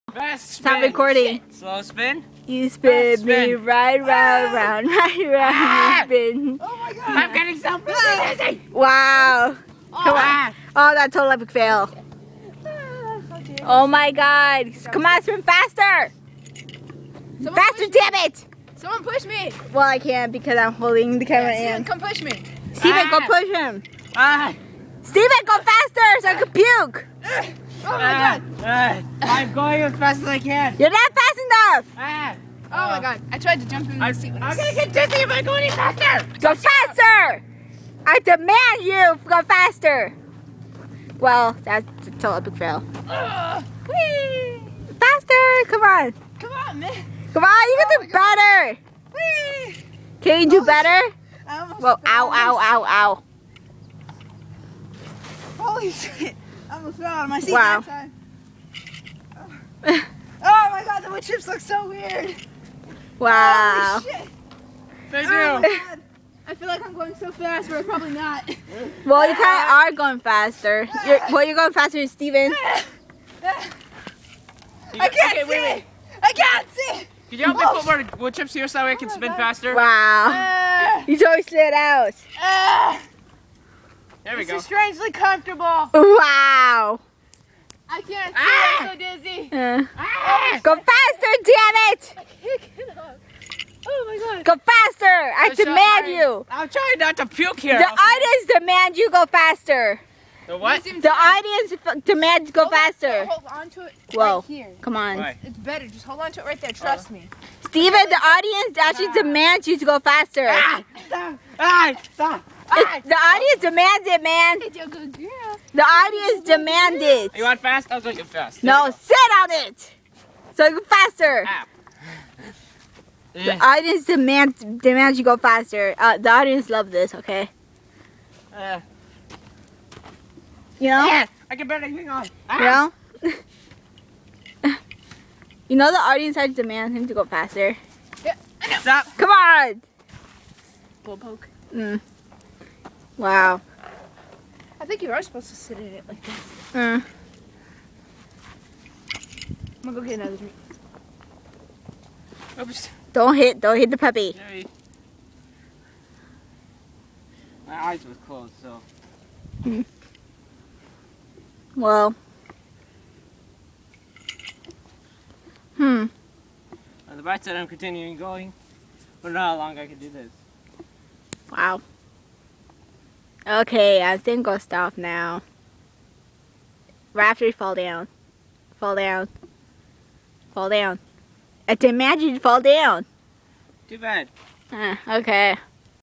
Spinning Chairs? 0.0
Still at the park and haveing a blast! :D I'm with my bestfriend/sister Piper, my brother Steven, Mydog/sister Jadis at the park.